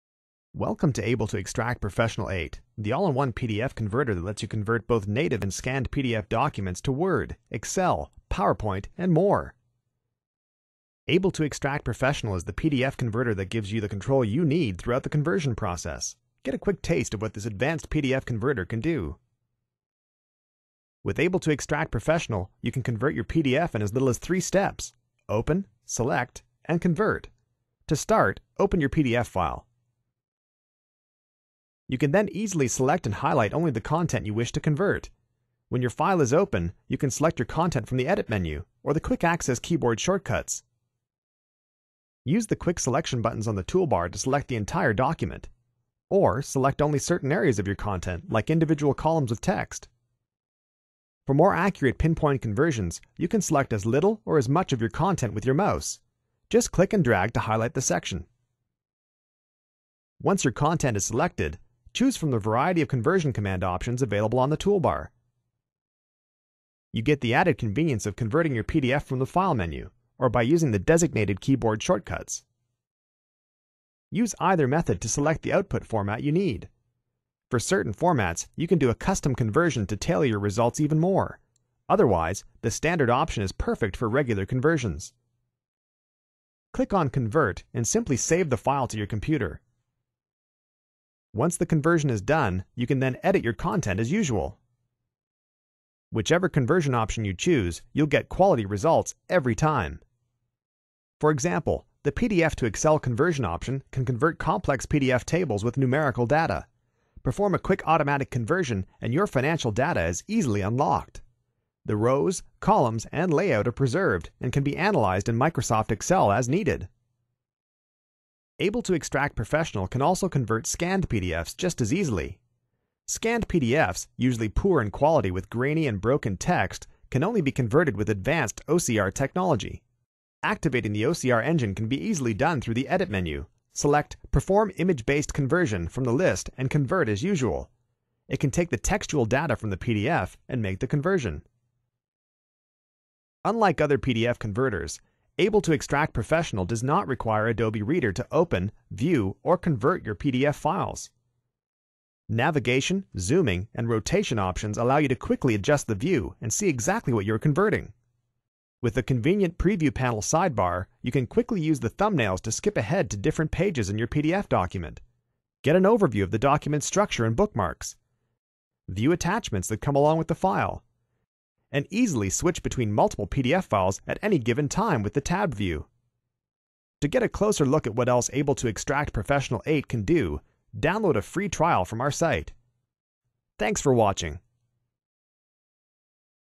Able2Extract Professional 8 0 42 0 Eng + Patch
Able2Extract PDF Converter cộng với nhận được OCR để chuyển đổi tự nhiên (bản địa) và scan PDF sang Excel, PDF sang Word, PDF sang PowerPoint, PDF để xuất bản, PDF sang hình ảnh (JPEG, TIFF), PDF sang Autocad và PDF sang văn bản. Able2Extract Professional chỉnh sửa, sửa đổi và phân tích PDF đơn giản và nhanh chóng.